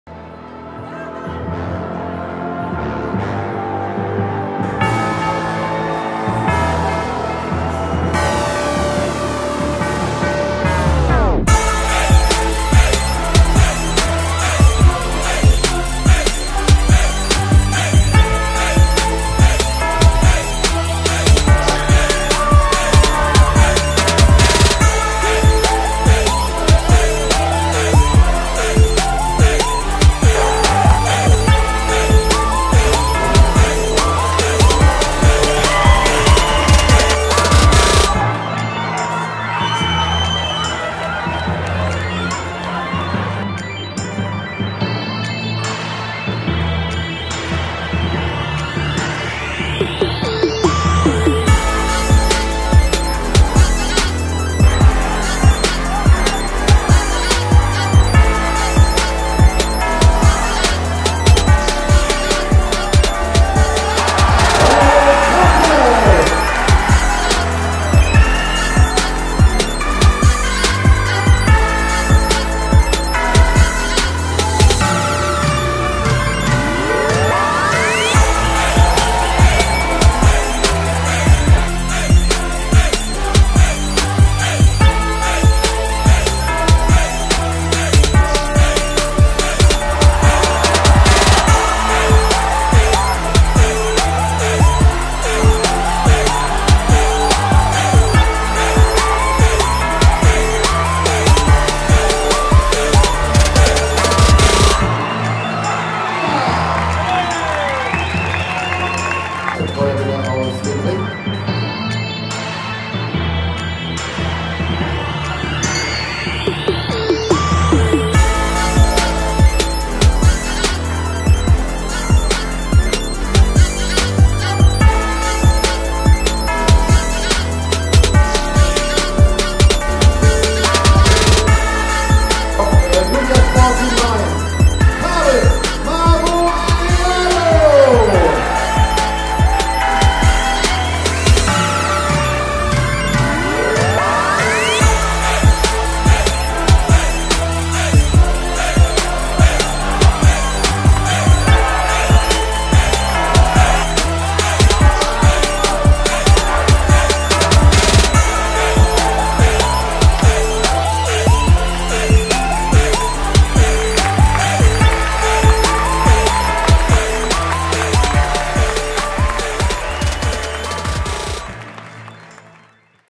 Nubia Fight Night Trailer vom 03.09.2016
Nubia-Fight-Night NO.4
Spannende Kämpfe im MMA,BOXEN,K-1 und Muay Thai organisiert vom dem Nubia-Sports Fitness und Kampfsportcenter aus Mainz-Kastel, Bad Kreuznach,Wiesbaden und Mainz .